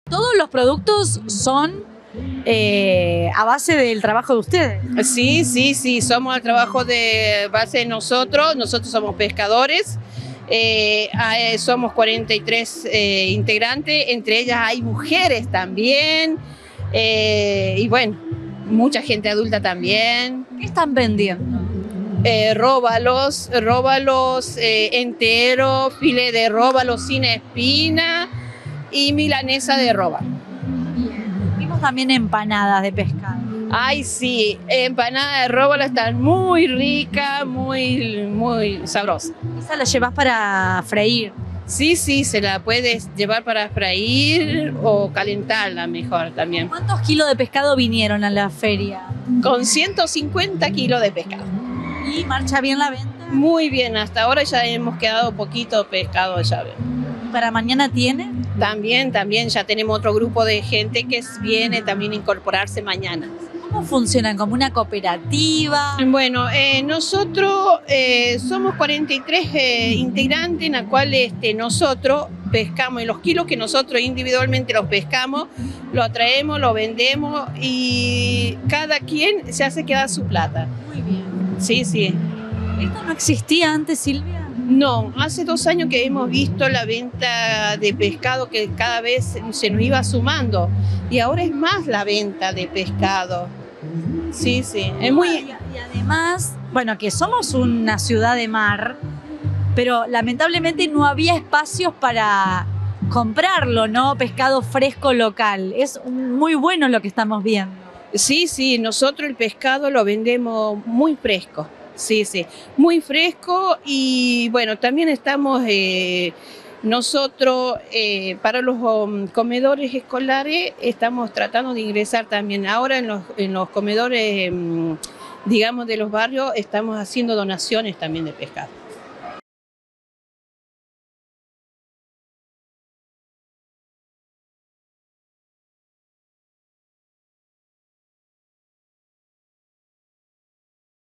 VENDEN PESCADO FRESCO A PRECIOS POPULARES POR SEMANA SANTA
La pesca artesanal es la única permitida en el Partido de La Costa, porque permite la regeneración de las colonias de peces y no daña el lecho marino, convirtiéndola en una actividad sustentable que no tiene impacto ambiental.